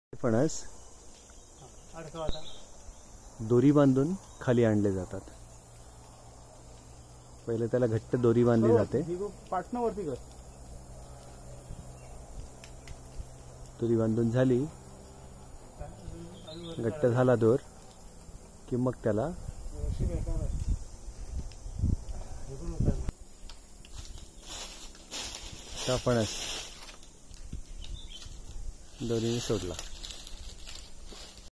How to pluck Jackfruit and safely bring it to down from tree
Jackfruit is heavy fruit and one just can't bring it down like mango so one should be careful while bringing it down. In Marathi it is called kava of rope on jackfruit to tie it down and then slowly bring it down.